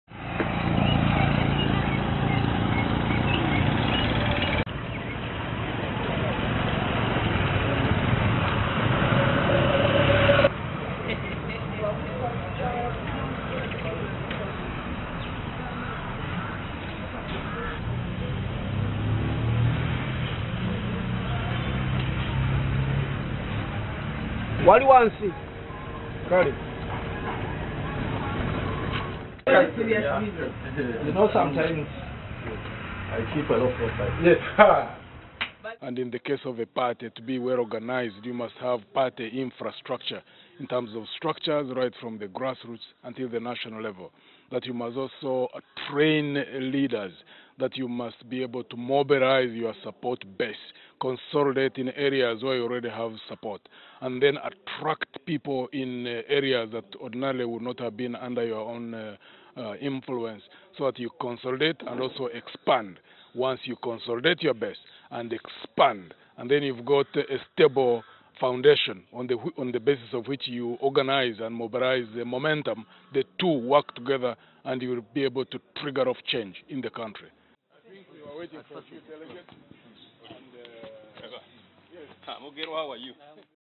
Muntu kicks off FDC flagbearer campaign in Hoima
Former army commander, Maj Gen. Mugisha Muntu has began his campaigns in the western region district of Hoima. The FDC party president met all Bunyoro FDC delegates and implored them to strengthen grassroot branches if the party is to win the 2016 election. Muntu is running against Col.Kizza Besigye for the FDC party flag-bearer position.